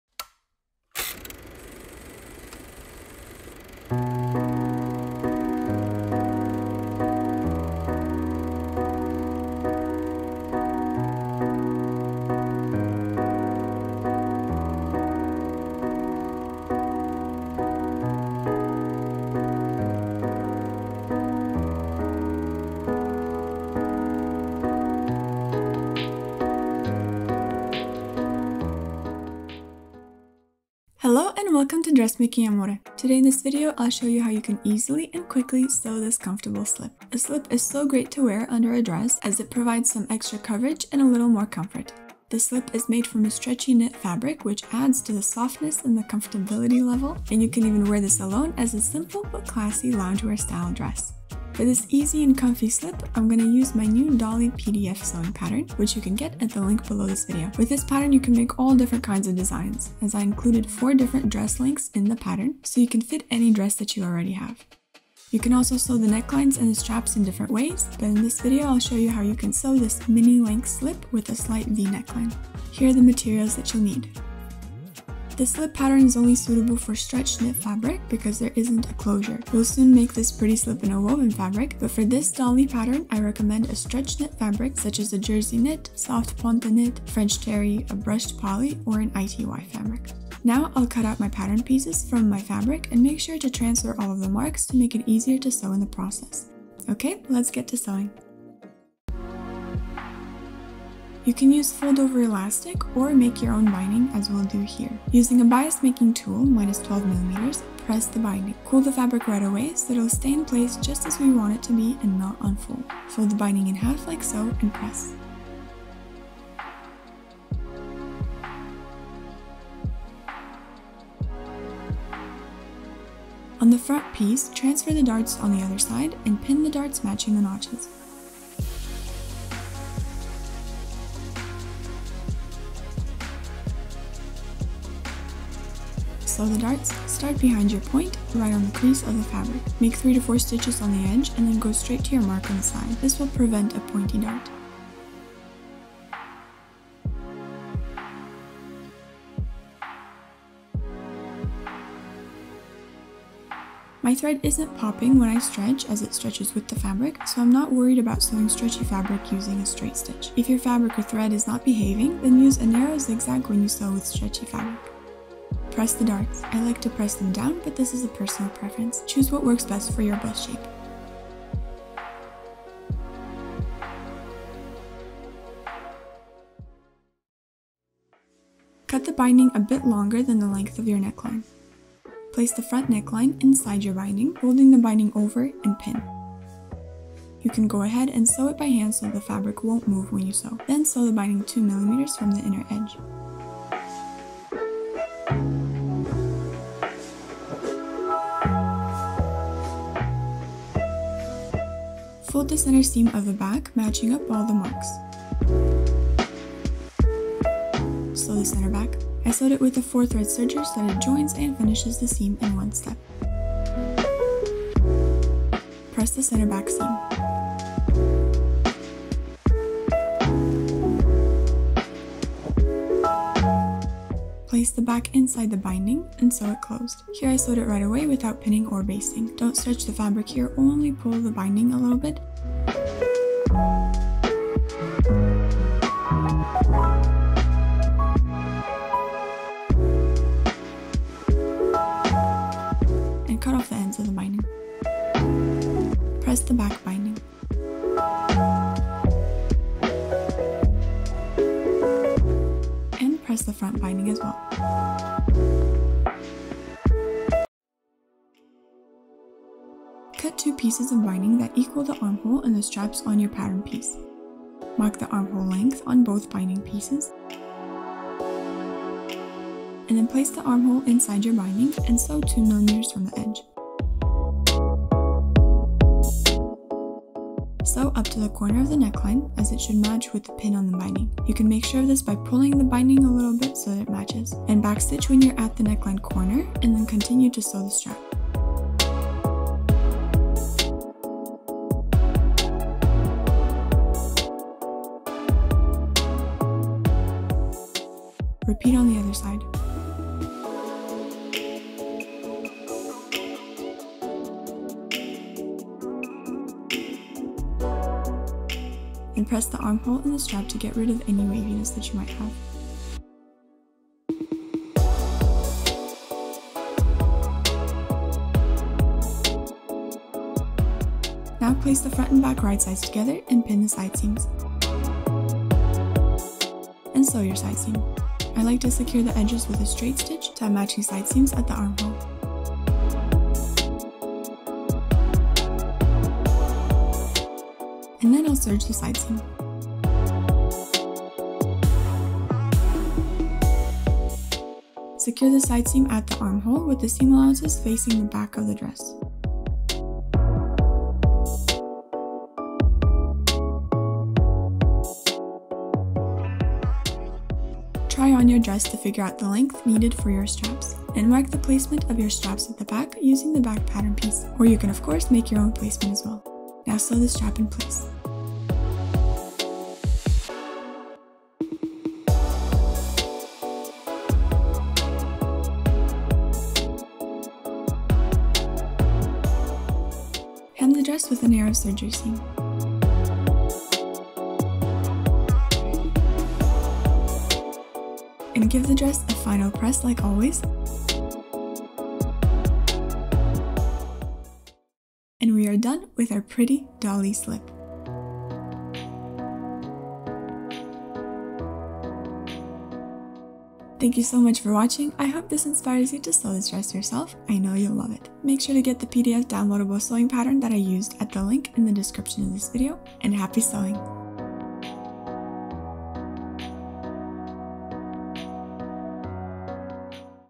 DIY SLIP DRESS - Stretch Knit Slip + PDF Sewing Pattern
Today in this video, I’ll show you how to easily and quickly sew this comfortable slip.
I’m using my Dolly slip dress PDF downloadable sewing pattern, get it

The materials you’ll need for this top are:
- 0.8-1.5 m or 0.9-1.65 yds of stretch knit fabric
- Fold-over elastic 3 m x 1-1.5 cm (5/8” wide)
- Bias-making tool to make your own binding
- machine needle for knit fabric, such as ballpoint, jersey, stretch
- matching thread
- glasshead pins
- scissors
- measurement tape
- Dolly pattern

This pattern is only made for light to medium weight stretch fabric - at least 5% elastane is recommended.
This would be something like a French Terry, Rayon or Cotton Jersey, Double Brushed Poly, Rayon or Bamboo Modal, or a Ribbed Knit. In this tutorial I’ll be using a Rayon Jersey.

Sewing tools used:
Double stick fusible tape
Glasshead pins
Marking tool
Table-top Ironing board
Wooden Tailor’s Clapper
Sewing magnet

Recommended sewing machines:
Janome MOD30
Janome 740DC
Juki M0644D Serger